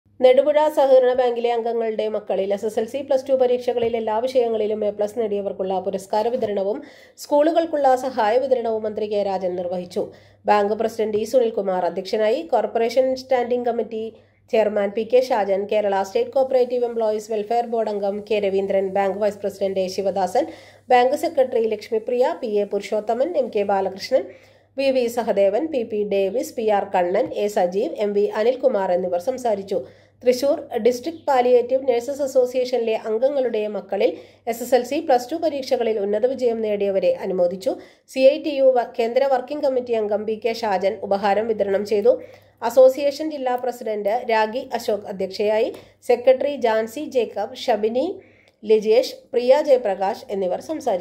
SAHAKARYAM NEWS#latestnewstoday
നെടുപുഴ സഹകരണ ബാങ്ക് വിജയികളെ ആദരിച്ചു